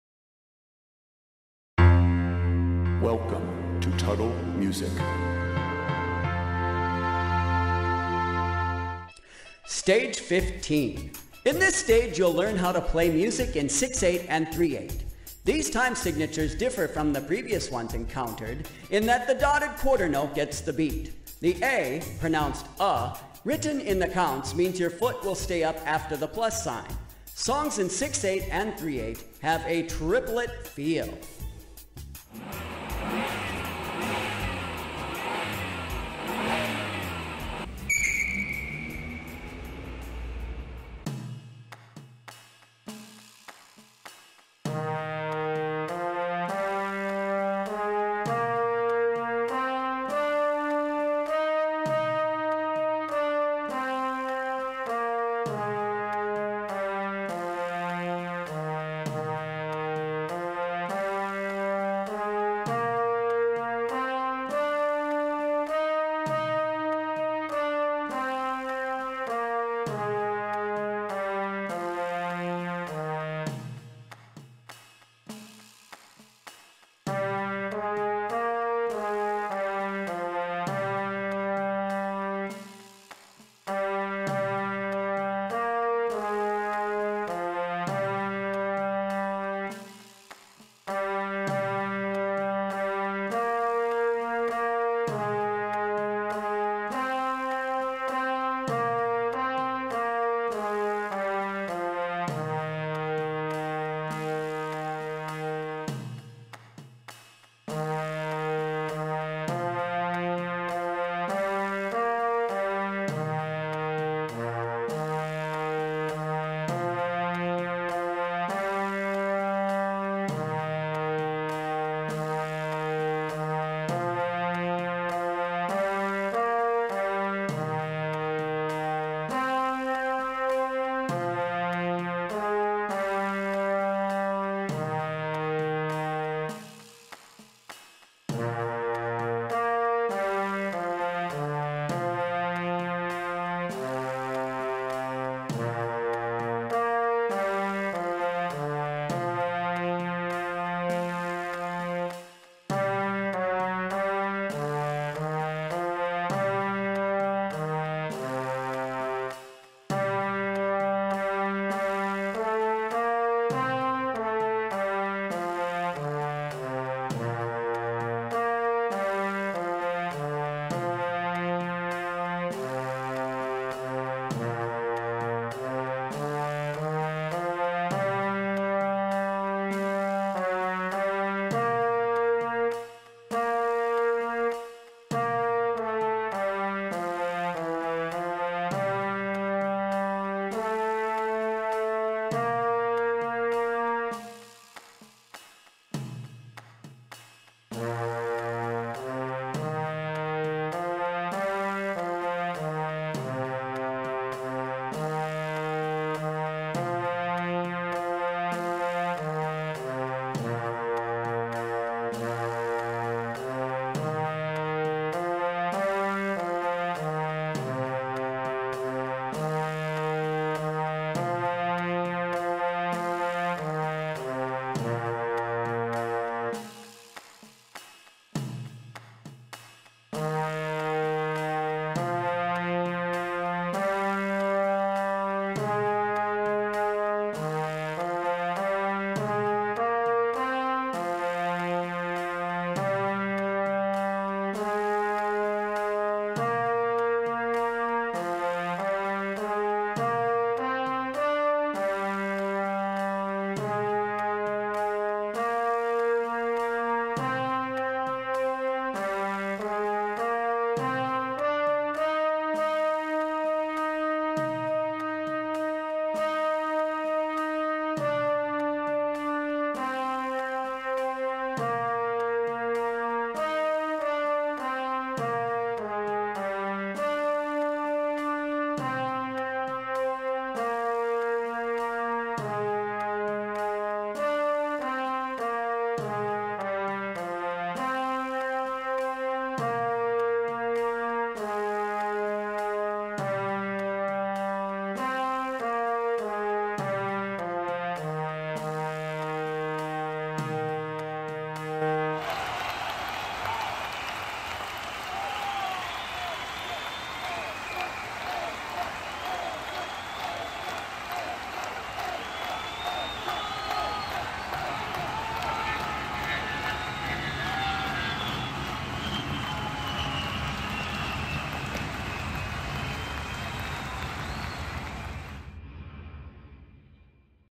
STAGE 15 FRENCH HORN
In this stage, you will learn how to play music in 6/8 and 3/8.  These time signatures differ from the previous ones encountered in that the dotted quarter note gets the beat.  The 'a' written in the counts means your foot will stay up after the plus sign.  Songs in 6/8 and 3/8 have a triplet feel.